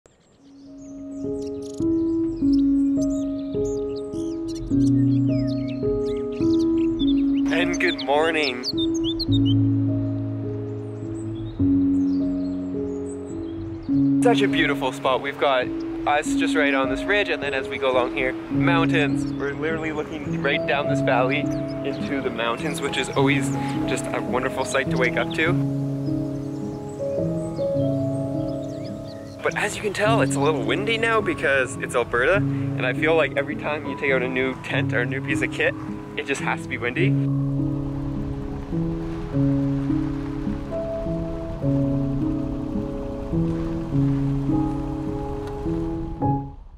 NIGHT OVERLANDING ADVENTURE - Subaru's and a GREAT VIEW! Teaser #2
Welcome to the world of overland camping! In this second teaser for our night overlanding adventure, we'll be exploring the wonders of Subaru's and a view!

In this video, we'll be testing out our new root top tents, as well as exploring some of the best overland routes in the Canadian Rockies. This adventure is going to be exciting and we can't wait to share it with you! Watch this channel for more updates!